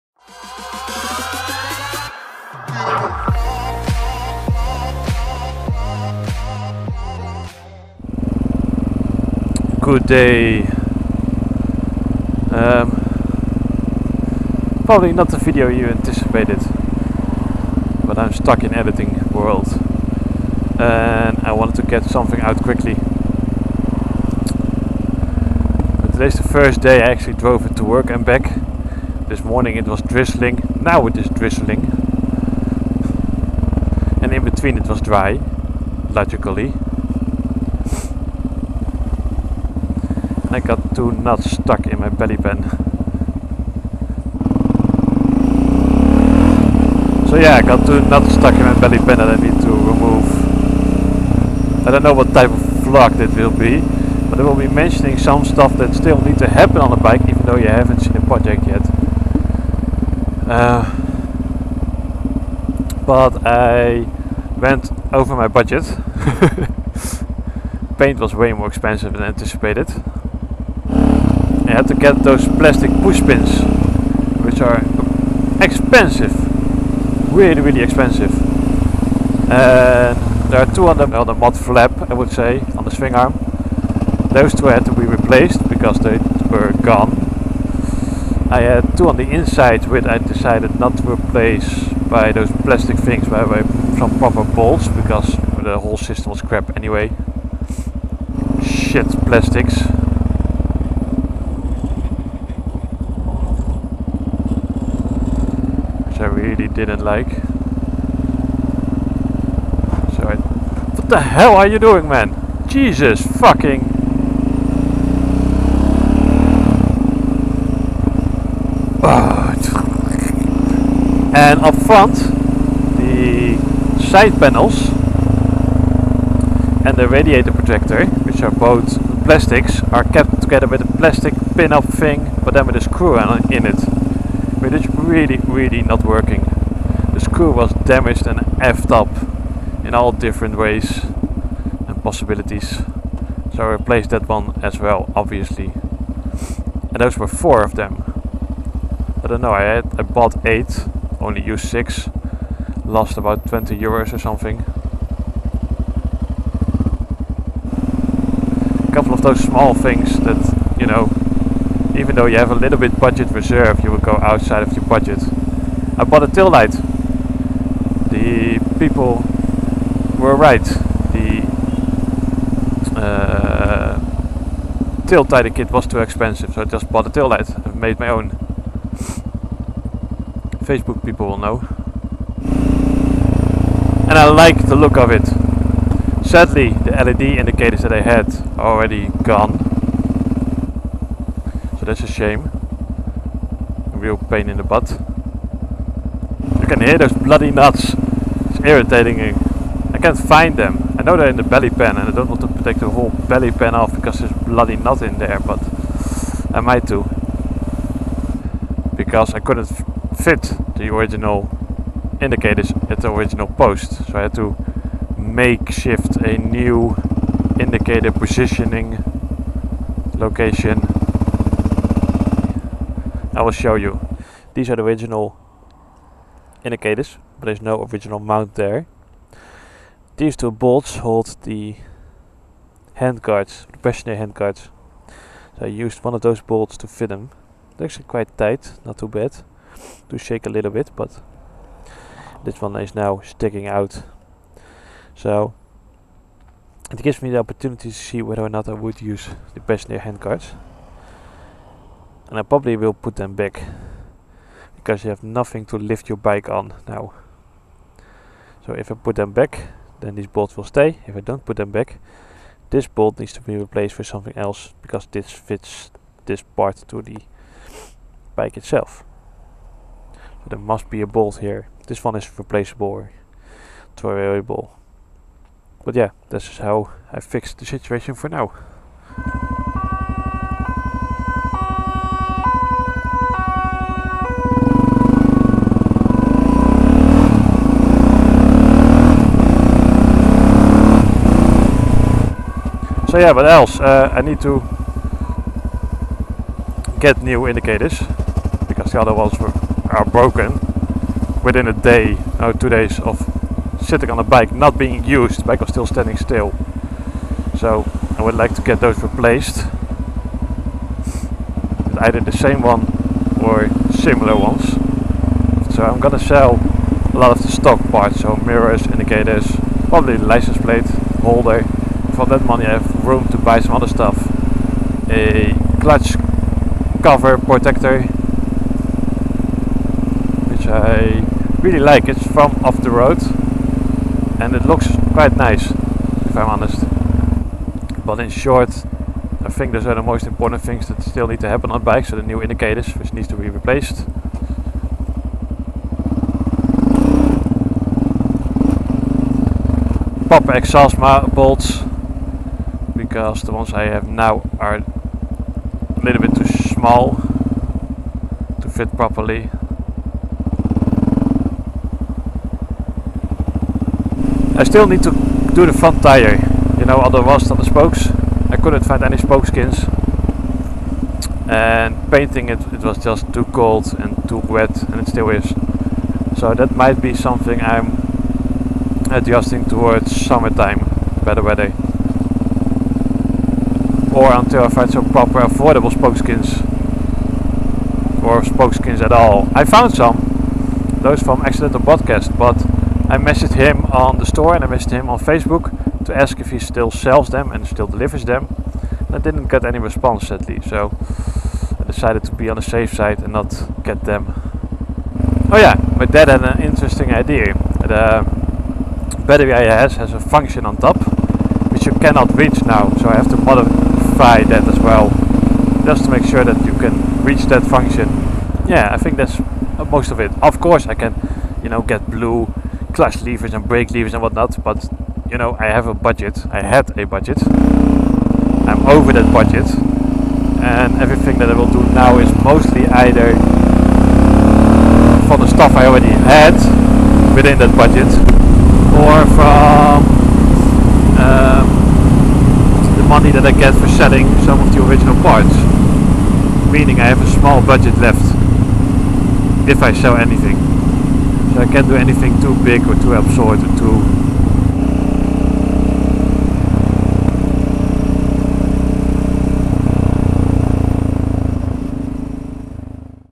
What will the Future bring for Yamaha
So yeah, the future plans for the XT.
Or nearby ideas... ;)

Equipment:
Main camera:  Drift Hd 1080p with the Drift Mic

Software:
MAGIX Movie Edit Pro (also called: MAGIX Video Deluxe Plus)

GEAR:
Helmet is a Icon Variant Big Game Helmet

MOTORCYCLE:
Motorcycle in the video:
Yamaha XT660R from 2004.